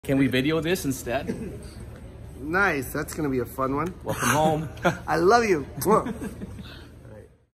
Jokoy sends his love
Jokoy is back at The Theatre Solaire Jan 11, 2020. His show sold out within 3 hours after tickets went on line.